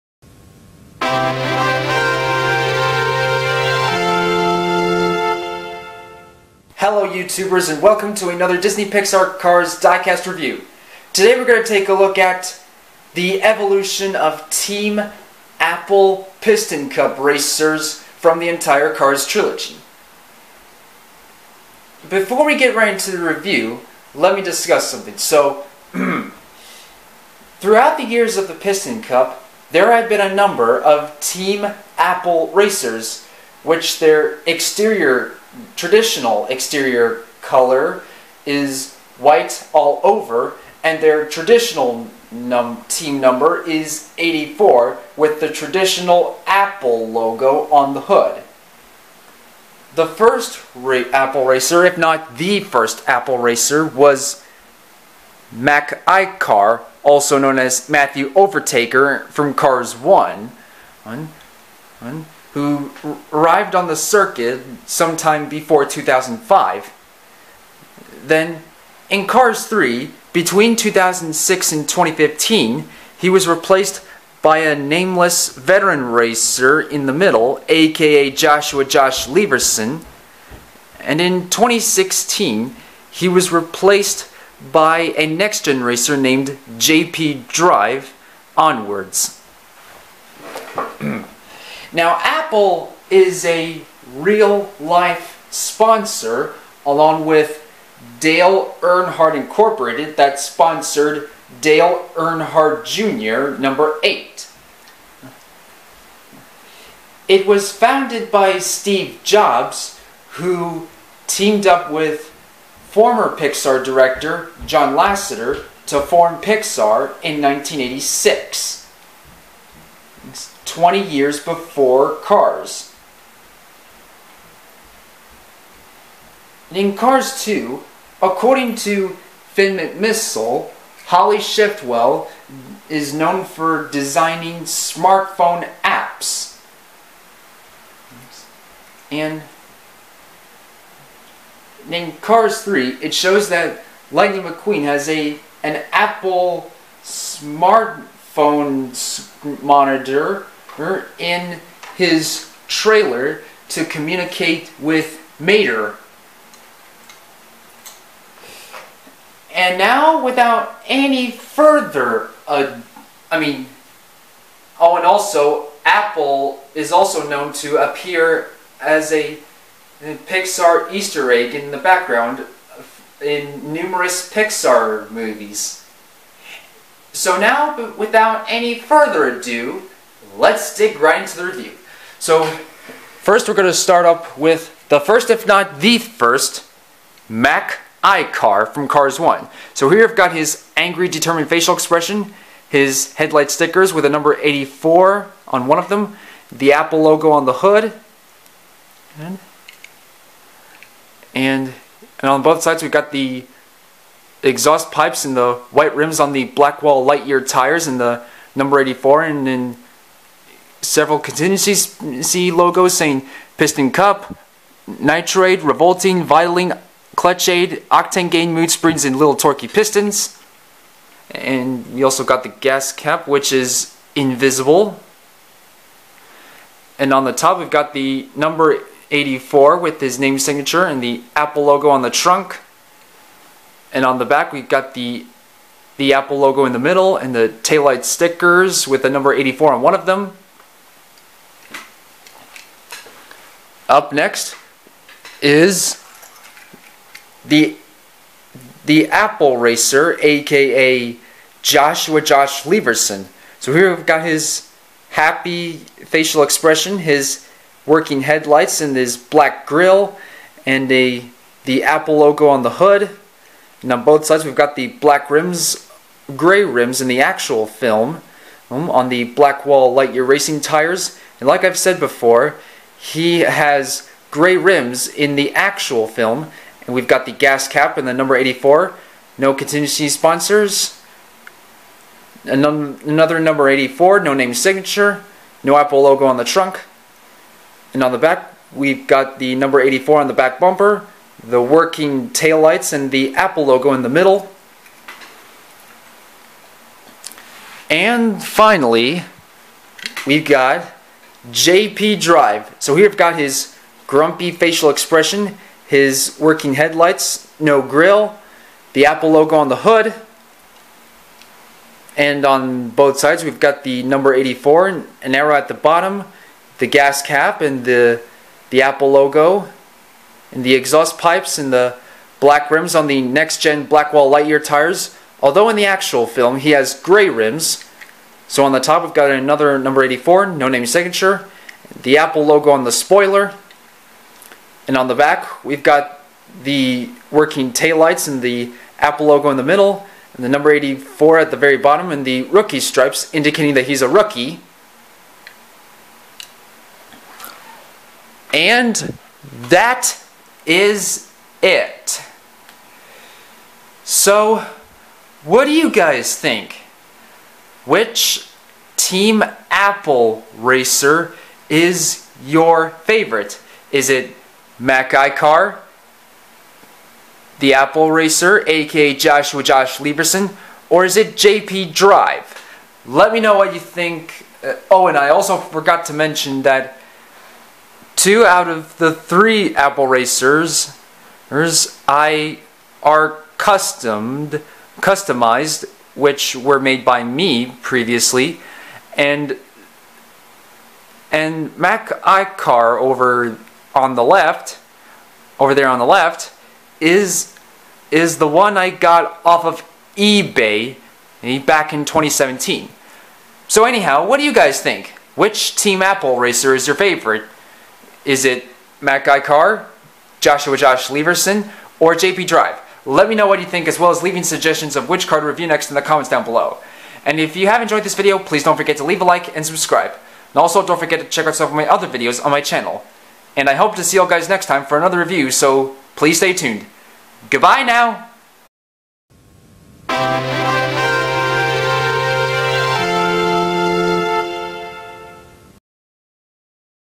Disney Pixar Cars Team Apple Piston cup racers (Mac, Joshua & JP) diecasts
Hello everybody & today, we're gonna take a look at The Evolution of Team Shiny Wax Piston cup racers (Mac ICar, Joshua Josh Leverson & Conrad Camber).
Cars belong to Disney/Pixar.